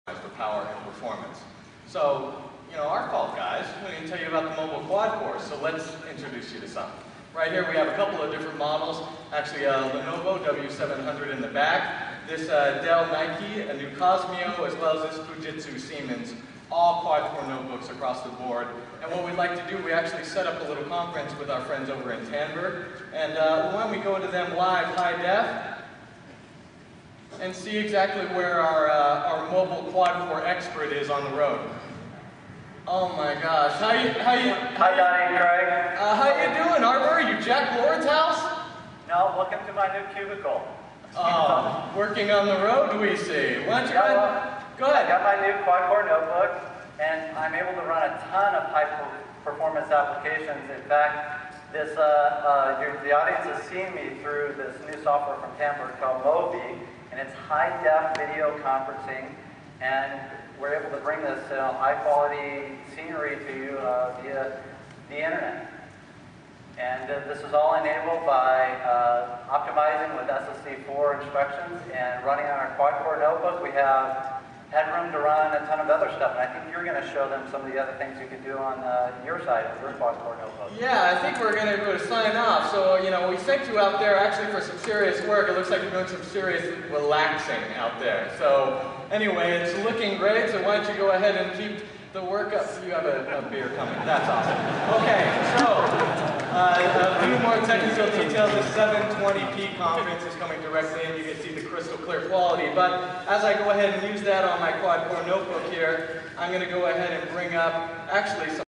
Tandberg Demo @ IDF2008
Nice demo of the new Mobile Quad Core chips from Intel running a new Tandberg service.  May not come through on my video, but the quality was awesome.